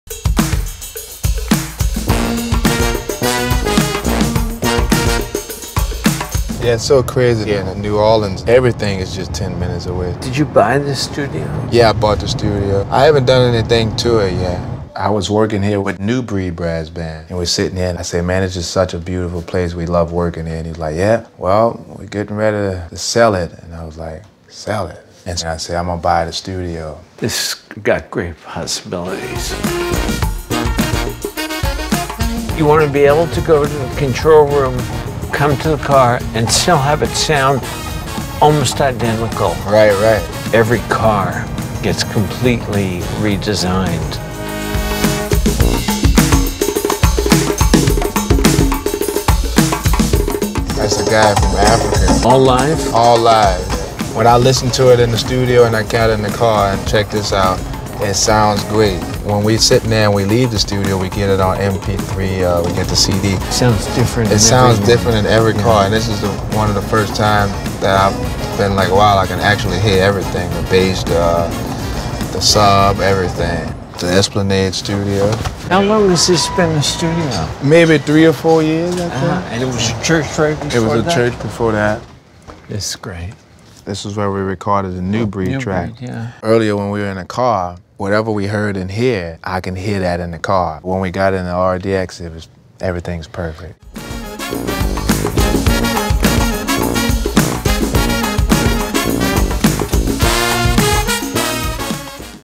Acura – Jazz Fest – ELS – Trombone Shorty
NOLA native son, Trombone Shorty, and Grammy award-winning engineer, Elliot Scheiner, take a break from Jazz Fest to tour New Orleans in the Acura RDX. The two music icons talk studios, sound, cars and the audio system that bring all three together, ELS Studio Premium Audio. Stops include Esplanade Studios, home too many of the area’s most notable musicians and Shorty’s new recording studio, Buckjump.